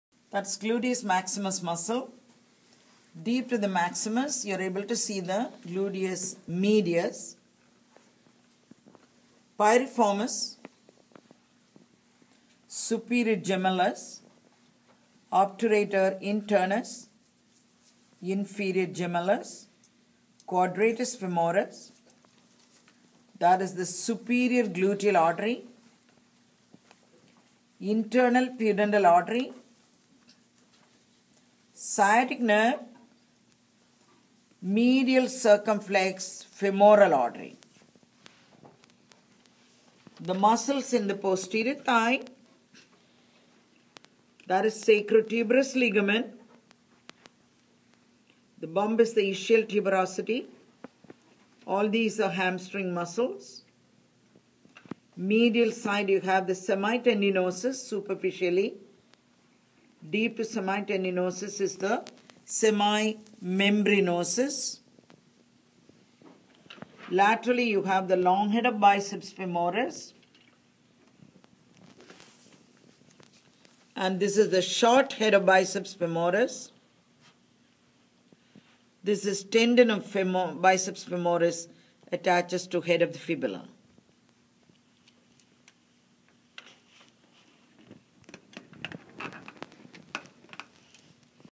Structures Deep to Gluteus Maximus -- Explanation over anatomical models
Source of Video- Dissector manual- 3 volumes DVD- by jaypee brothers publications.

Please don't hesitate to be in touch.

Assistant Professor.